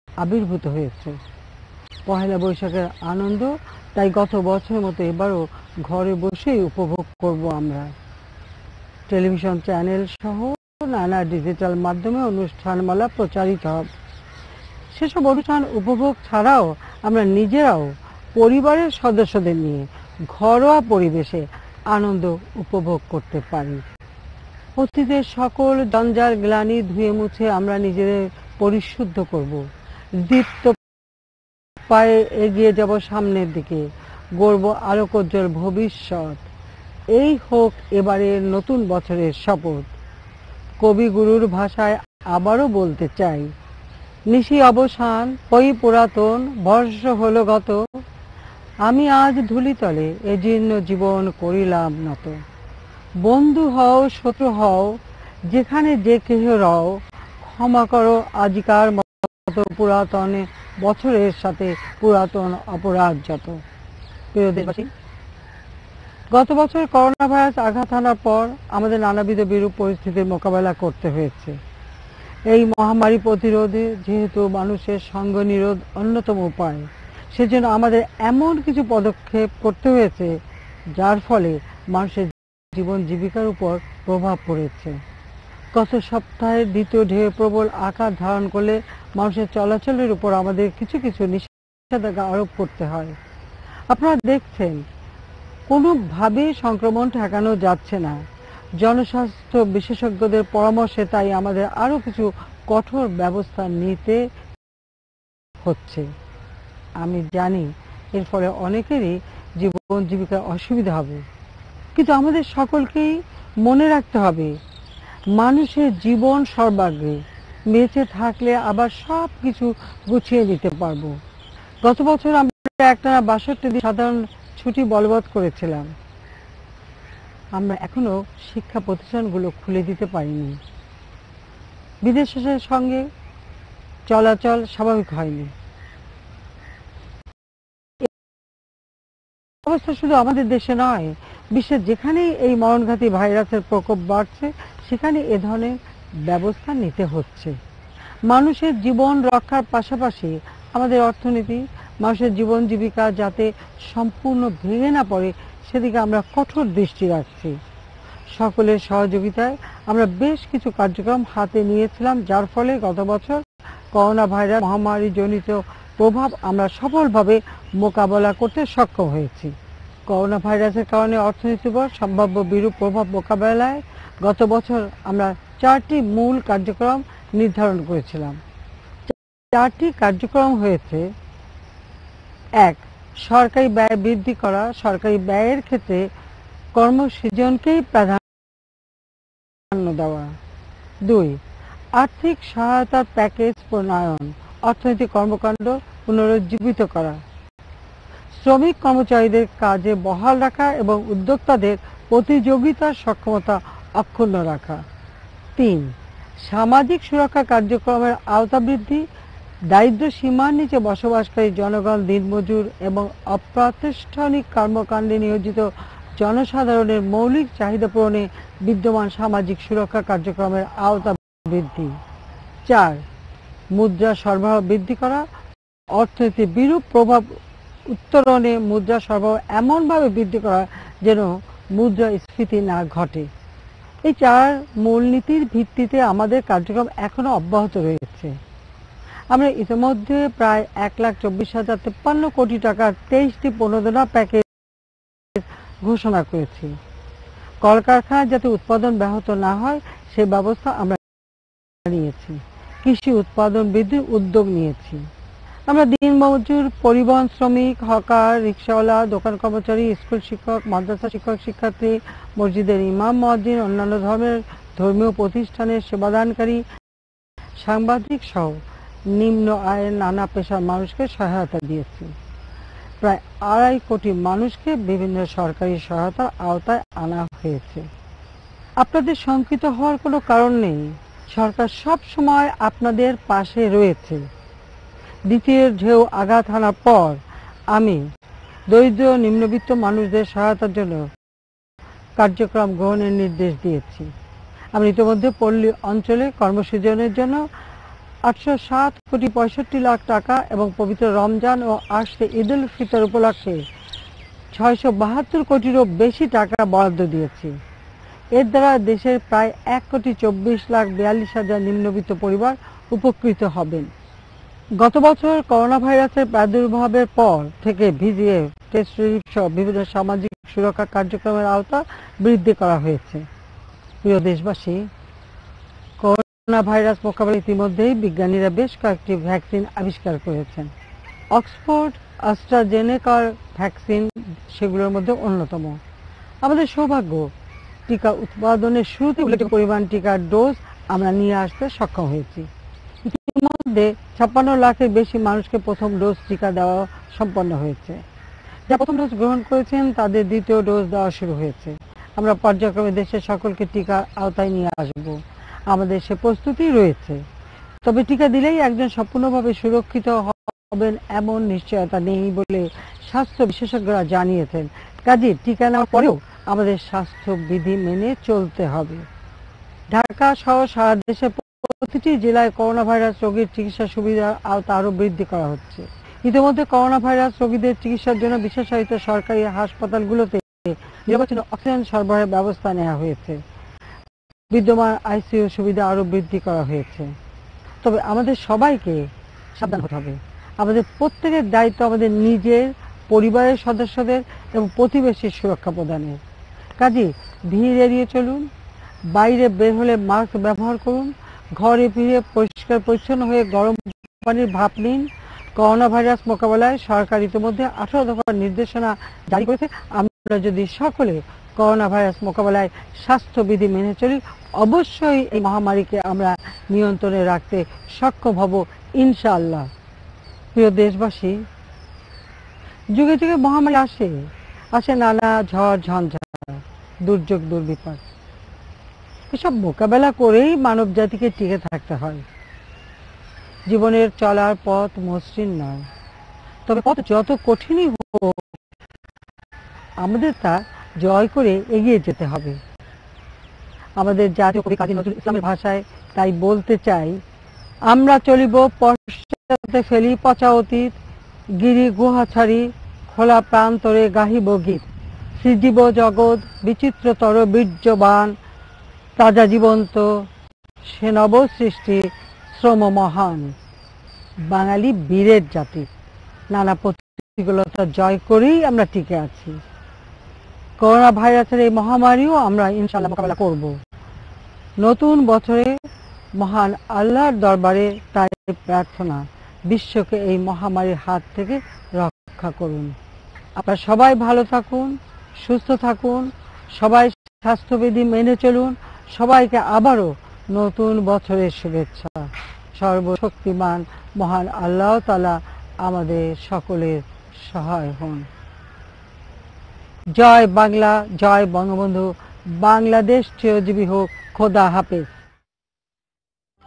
জাতির উদ্দেশ্যে মাননীয় প্রধানমন্ত্রী জননেত্রী শেখ হাসিনার ভাষণ।
গণপ্রজাতন্ত্রী বাংলাদেশ সরকারের মাননীয় প্রধানমন্ত্রী জননেত্রী শেখ হাসিনা বাংলা শুভ নববর্ষ ১৪২৮ উপলক্ষ্যে জাতির উদ্দেশ্যে ভাষণ প্রদান করছেন